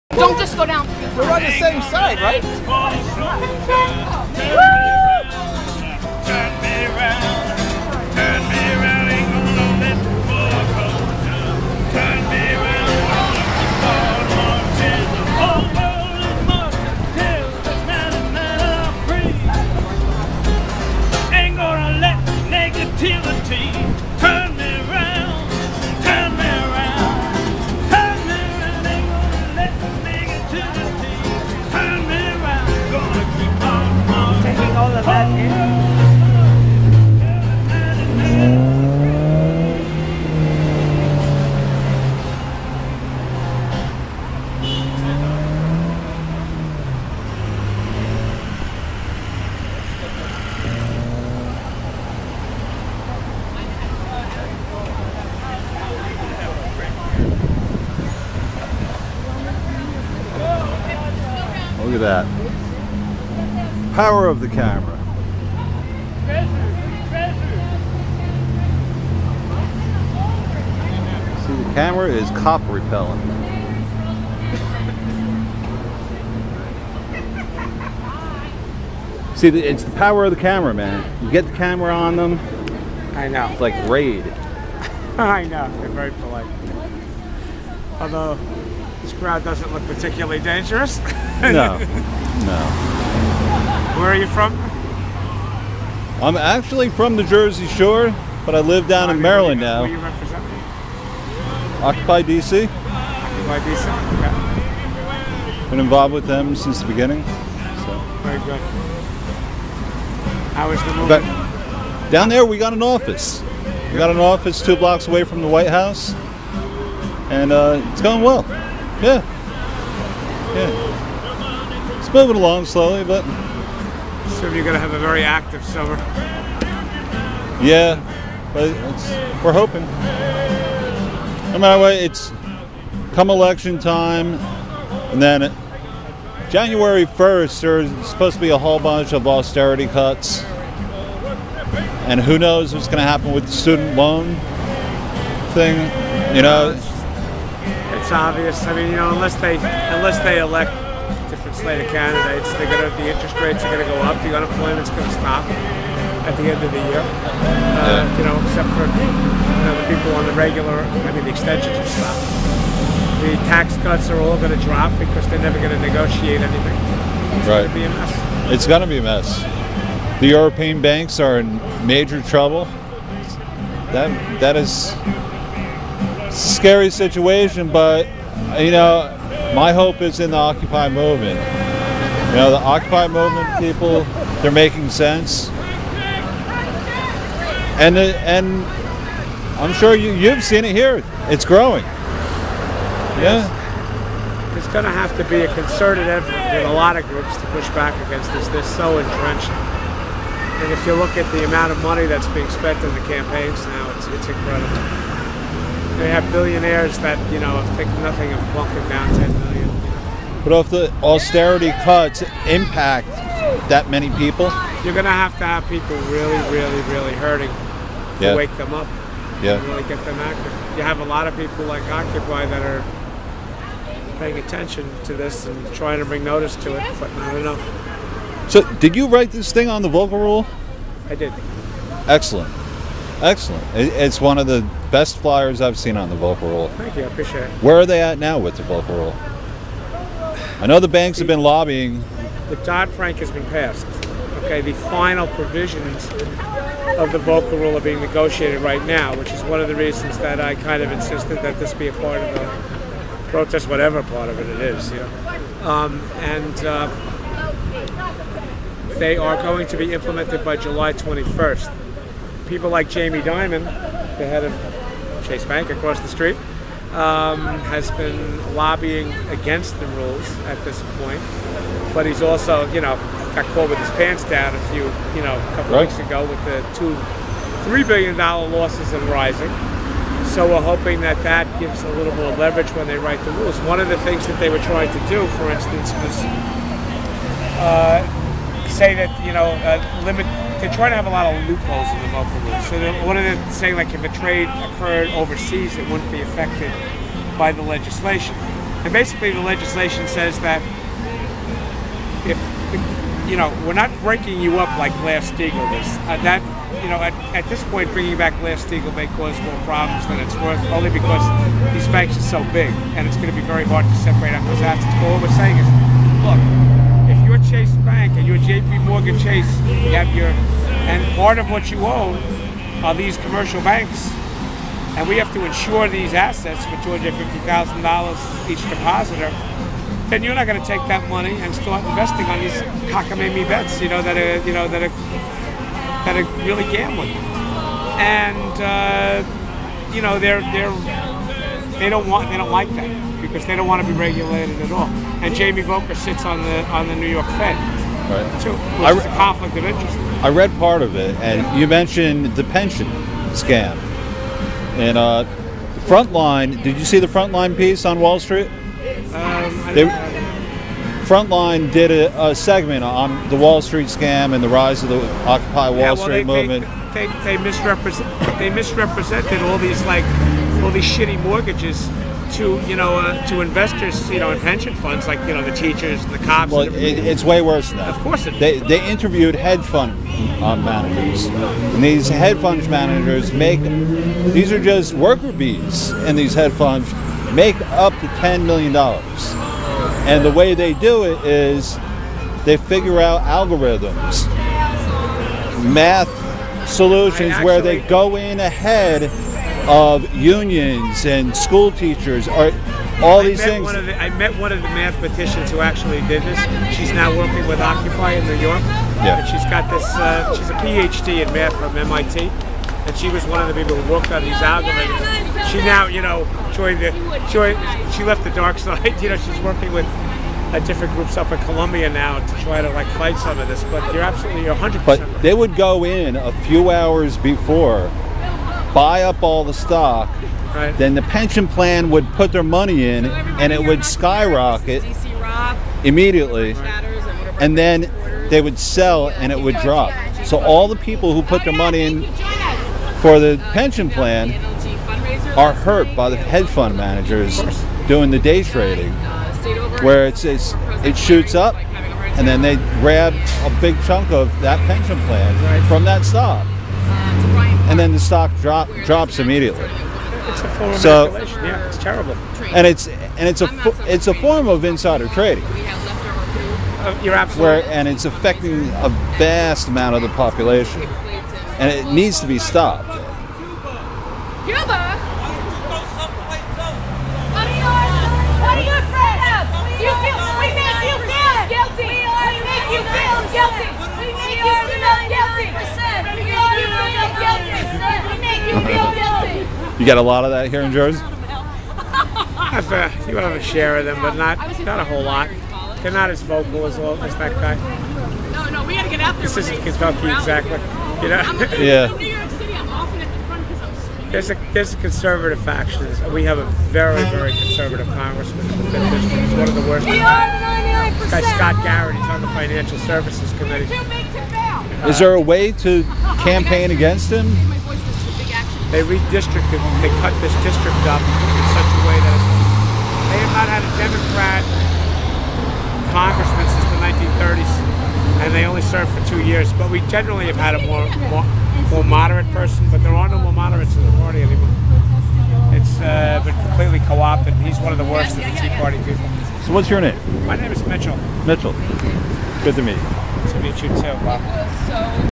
Occupy Bergen County June 2 2012 ~ It was a pleasure to join this North Jersey group
This is the Occupy Bergen County group with an action at Bank of America ~ Towards the end of the action the Bergen County police officers and other people complaining about the Occupiers seem to be allergic to the camera for some reason. Watch them scurry off as if I sprayed water on a litter of cats.